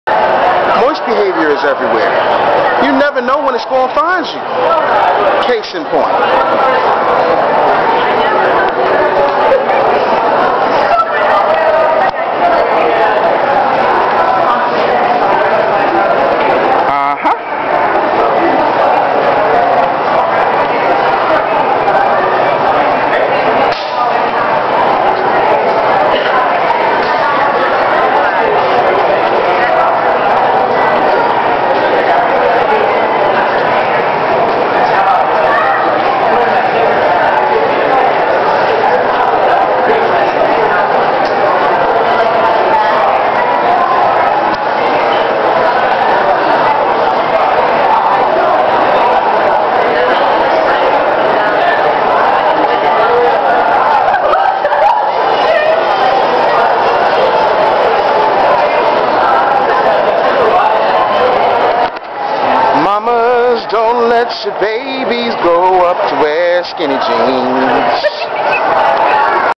Moist Dudes in Skinny Jeans Dancing
Tazz Rebukes Even MORE Moist behavior, as these kids are dancing at the train station wearing skinny jeans and moist outfits and accessories. Tazz exposes it and rebukes it in the name of the game.